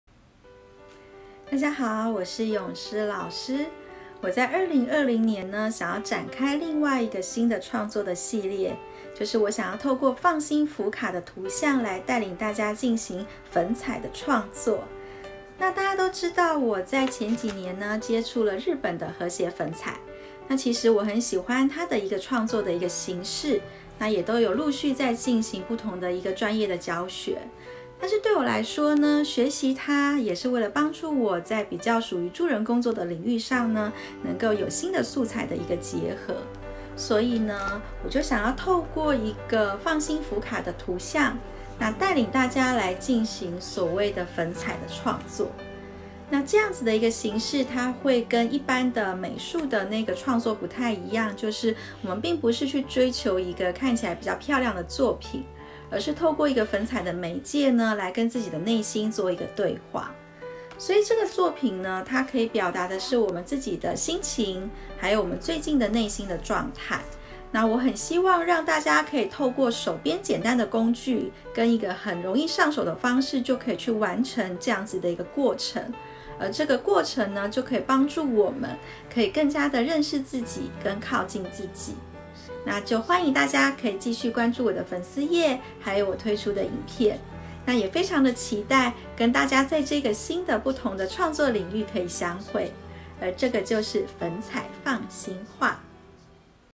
【粉彩放心畫～與你Fun心對話】故事緣起
2020年詠詩老師將開始一個新的創作，就是粉彩放心畫。
從2017年跟著日本創辦人細谷老師學習和諧粉彩，詠詩老師一直把這樣的工具跟精神放進助人工作當中。
但也因為如此，有別於只是為了創作出美麗的作品，更希望把和諧粉彩帶入心靈與藝術療癒的領域。
而透過Fun心福卡的圖像，希望讓不會畫畫的夥伴也可以透過手邊的工具，很容易就透過粉彩來表達內心的樣貌！
而這就是粉彩放心畫的由來囉！